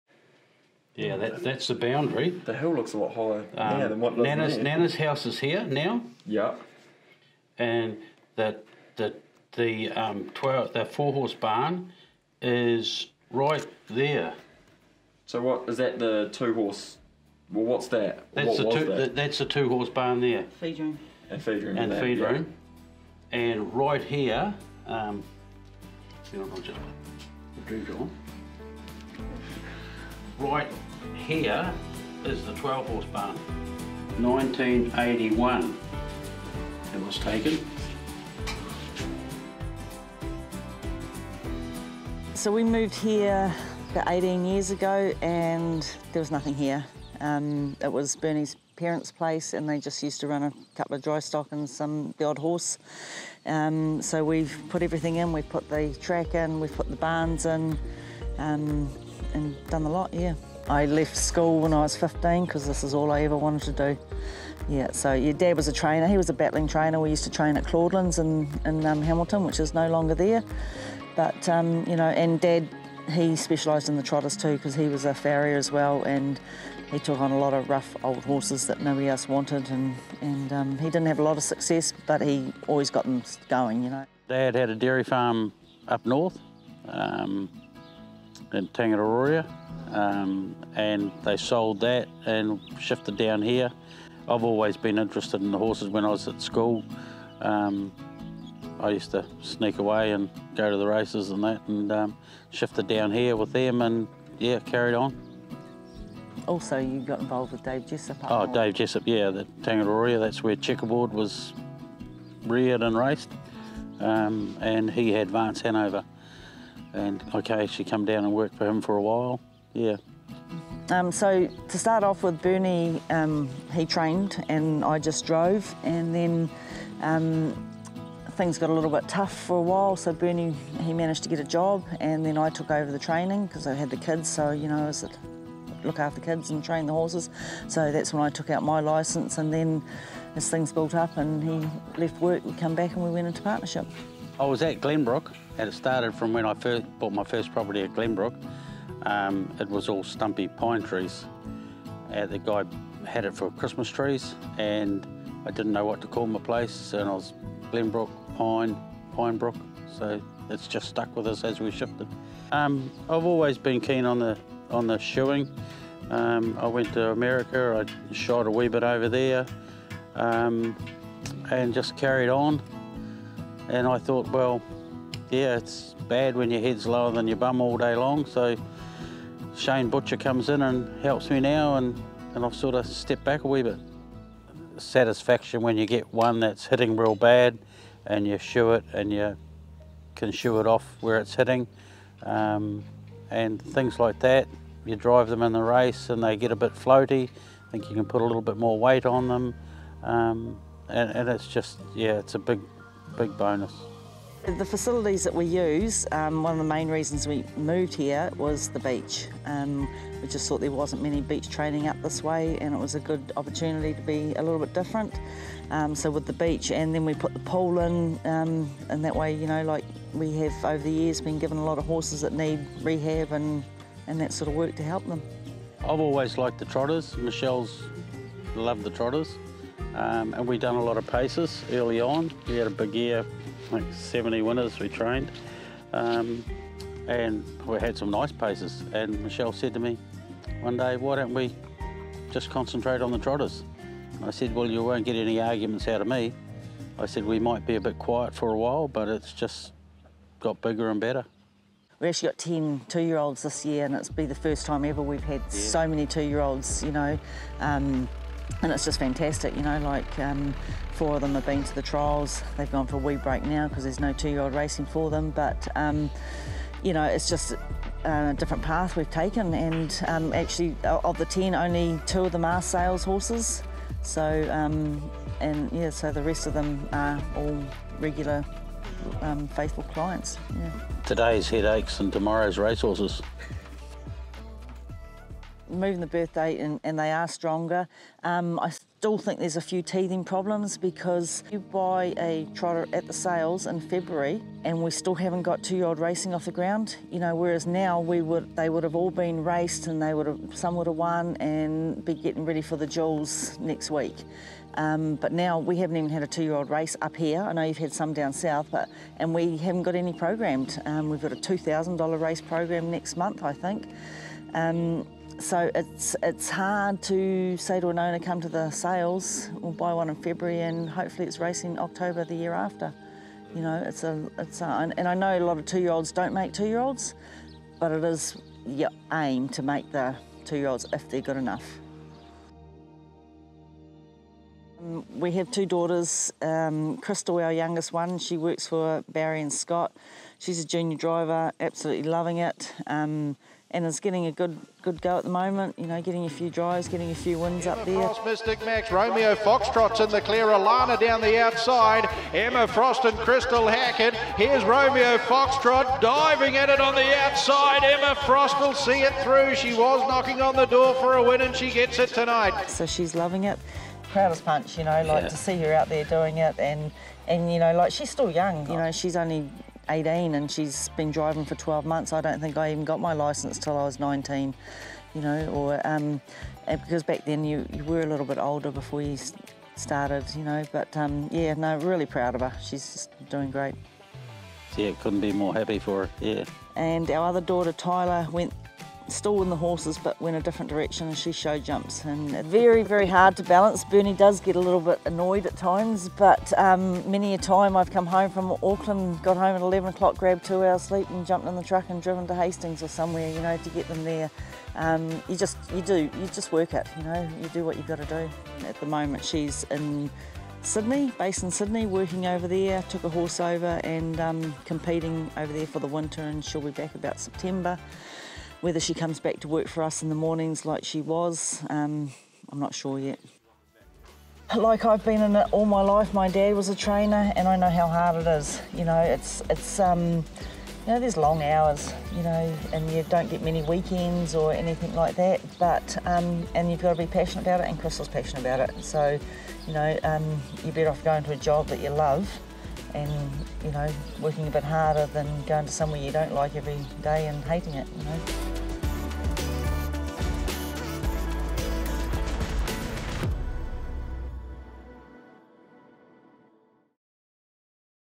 Michelle Wallis & Bernie Hackett | HRNZ Profile
Following a highlight worthy weekend it's a great time to catch up with Michelle Wallis and Bernie Hackett and get to know them.